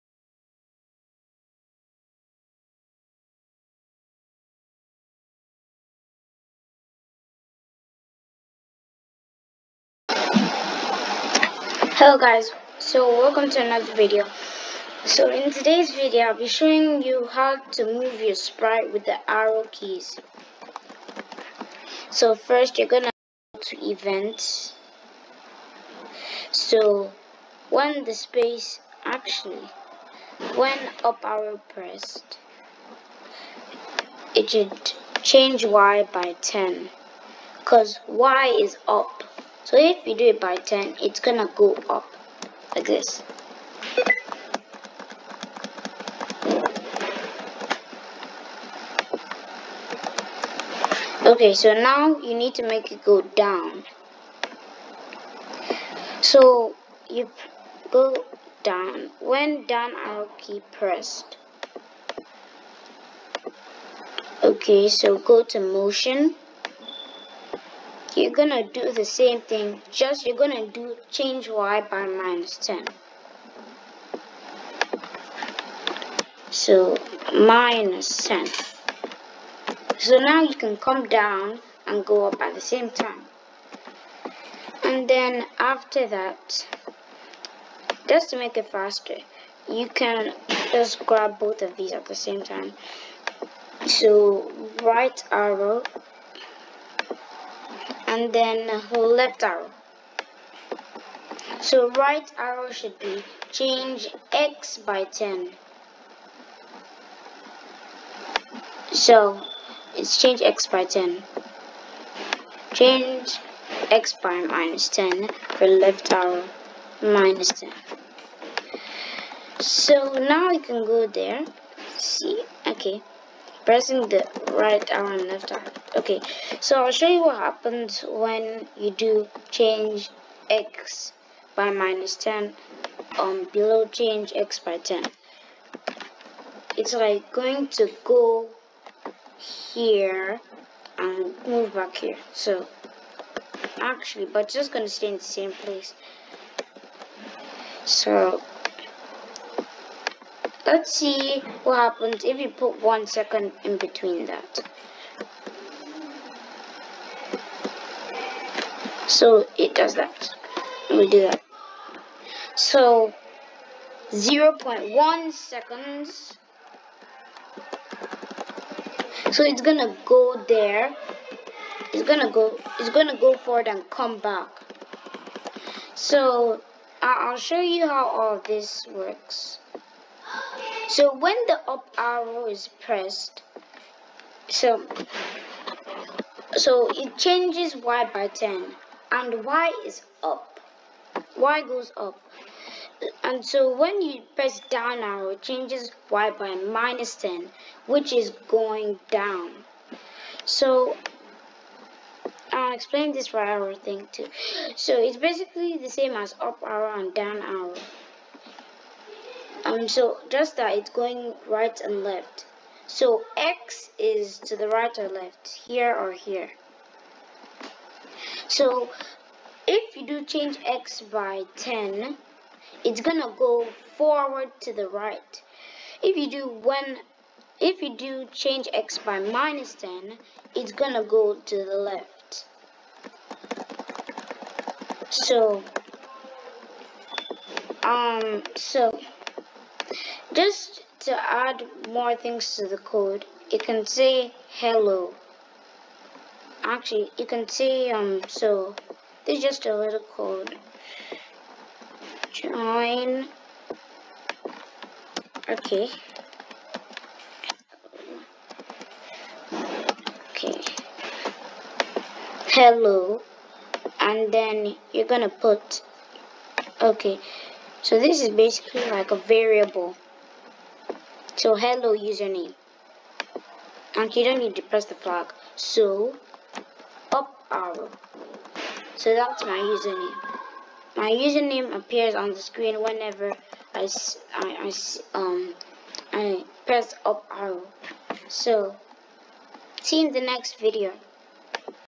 How to make A Sprite go up,down,right and left
IN This video, I will show you how to make Your sprite go up, down, right and left
Link to scratch
Link htttps://scratch.mit.edu
 My Channel: